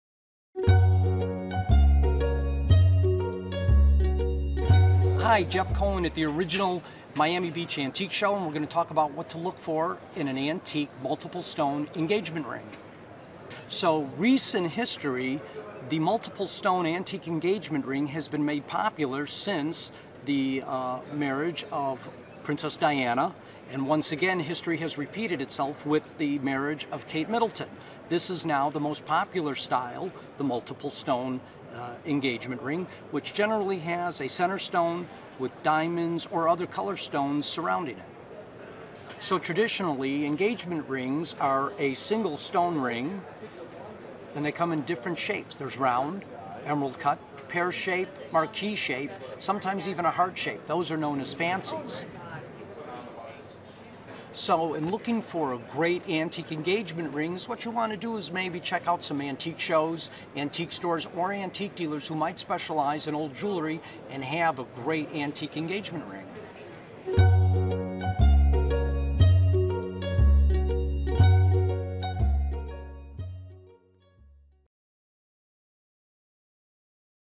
Multiple Stone Engagement Jewelry With Antique Settings
Multiple Stone Engagement Jewelry With Antique Settings. Part of the series: Antique Watches & Jewelry. Multiple stone engagement jewelry with antique settings are a very interesting topic that you're definitely going to want to learn more about. Find out about multiple stone engagement jewelry with antique settings with help from an experienced watch expert in this free video clip.